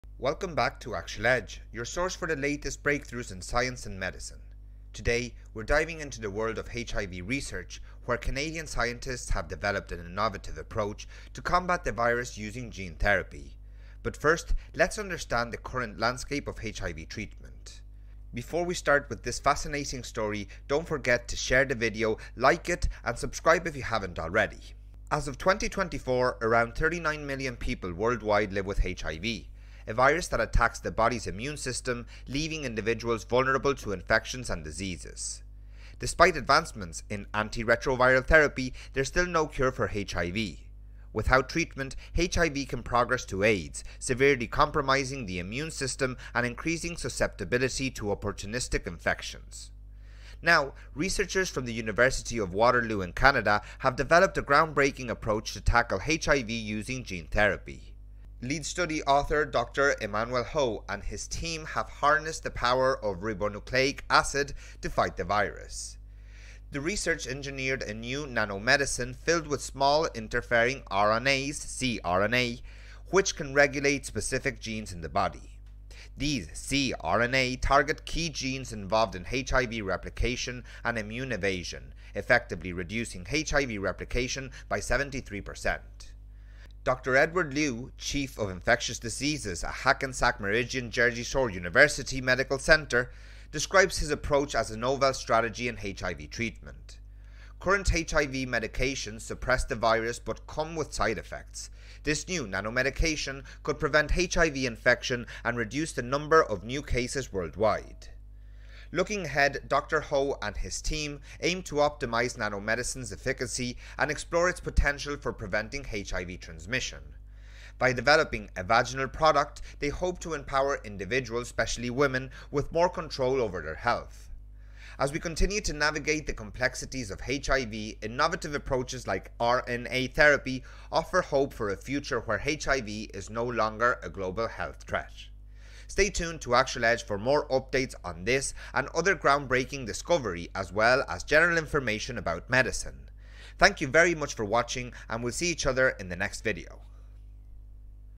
UPDATE 2024. More PROGRESS made in the CURE of HIV with RNA
The video explores groundbreaking research from the University of Waterloo, Canada, where scientists have pioneered an innovative approach to combat HIV using RNA therapy. By harnessing the power of small interfering RNAs (siRNAs), researchers target key genes involved in HIV replication, potentially offering a new avenue for treatment. Experts weigh in on the implications of this approach and its potential to transform HIV care, offering hope for a future where the virus is no longer a global health threat